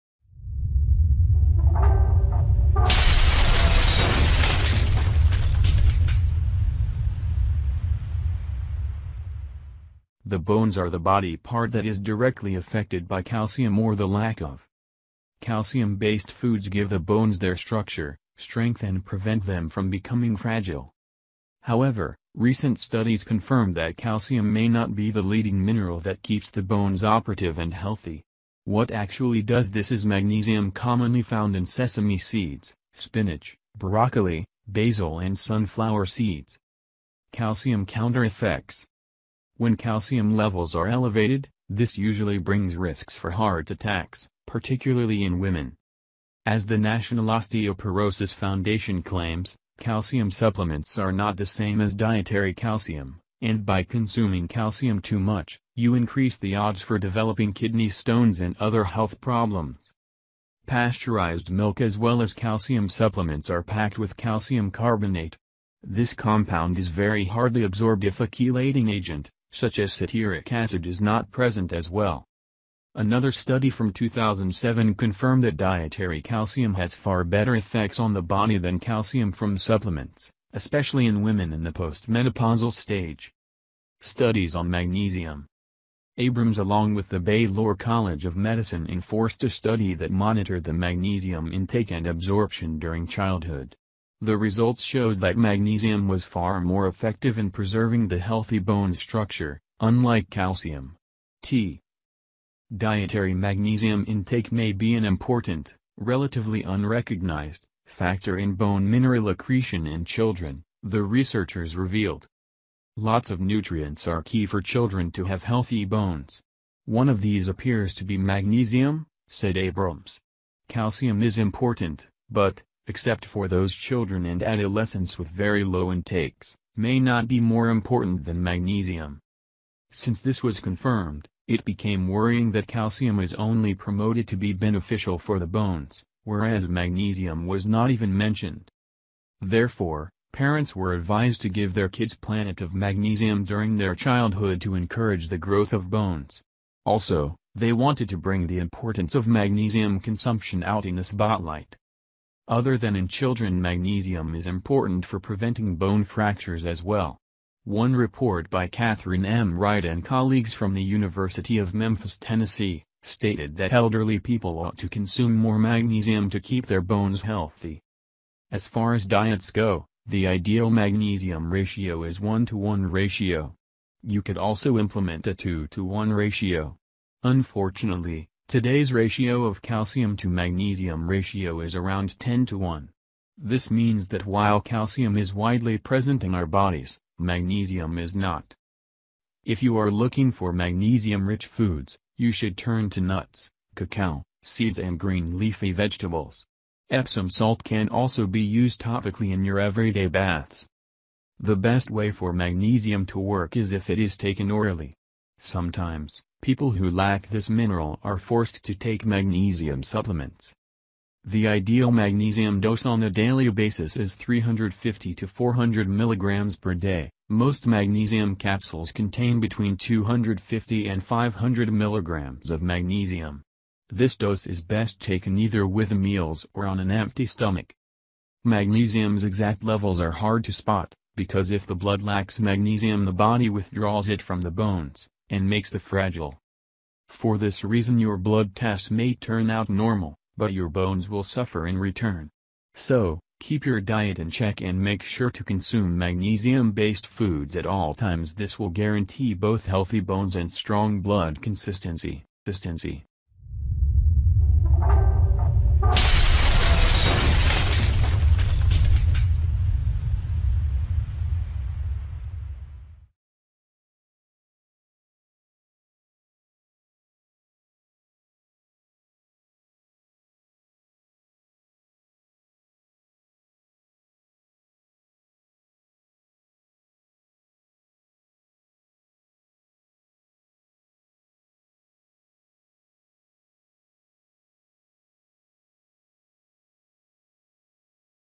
Researchers Make Surprising Discovery: Magnesium, Not Calcium, Is The Key To Healthy Bones
The bones are the body part that is directly affected by calcium or the lack of.

Calcium-based foods give the bones their structure, strength and prevent them from becoming fragile.

However, recent studies confirm that calcium may not be the leading mineral that keeps the bones operative and healthy. What actually does this is magnesium commonly found in sesame seeds, spinach, broccoli, basil and sunflower seeds.

Calcium counter-effects.

When calcium levels are elevated, this usually brings risks for heart attacks, particularly in women.

As The National Osteoporosis Foundation claims, calcium supplements are not the same as dietary calcium, and by consuming calcium too much, you increase the odds for developing kidney stones and other health problems.

Pasteurized milk as well as calcium supplements are packed with calcium carbonate. This compound is very hardly absorbed if a chelating agent, such as citiric acid is not present as well.

Another study from 2007 confirmed that dietary calcium has far better effects on the body than calcium from supplements, especially in women in the postmenopausal stage.

Studies on Magnesium.

Abrams along with the Baylor College of Medicine enforced a study that monitored the magnesium intake and absorption during childhood. The results showed that magnesium was far more effective in preserving the healthy bones structure, unlike calcium. T

“Dietary magnesium intake may be an important, relatively unrecognized, factor in bone mineral accretion in children,” the researchers revealed.

“Lots of nutrients are key for children to have healthy bones. One of these appears to be magnesium,” said Abrams. “Calcium is important, but, except for those children and adolescents with very low intakes, may not be more important than magnesium.”

Since this was confirmed, it became worrying that calcium is only promoted to be beneficial for the bones, whereas magnesium was not even mentioned.

Therefore, parents were advised to give their kids planet of magnesium during their childhood to encourage the growth of bones. Also, they wanted to bring the importance of magnesium consumption out in the spotlight.

Other than in children magnesium is important for preventing bone fractures as well. One report by Kathryn M. Ride and colleagues from the University of Memphis, Tennessee, stated that elderly people ought to consume more magnesium to keep their bones healthy.

As far as diets go, the ideal magnesium ratio is 1:1 ratio. You could also implement a 2:1 ratio. Unfortunately, today’s ratio of calcium to magnesium ratio is around 10:1. This means that while calcium is widely present in our bodies, magnesium is not.

If you are looking for magnesium rich foods, you should turn to nuts, cacao, seeds and green leafy vegetables. Epsom salt can also be used topically in your everyday baths.

The best way for magnesium to work is if it is taken orally. Sometimes, people who lack this mineral are forced to take magnesium supplements.

The ideal magnesium dose on a daily basis is 350-400 mg per day; most magnesium capsules contain between 250 and 500 mg of magnesium. This dose is best taken either with meals or on an empty stomach.

Magnesium’s exact levels are hard to spot, because if the blood lacks magnesium the body withdraws it from the bones, and makes the fragile.

For this reason your blood test may turn out normal, but your bones will suffer in return. So, keep your diet in check and make sure to consume magnesium-based foods at all times- this will guarantee both healthy bones and strong blood consistency.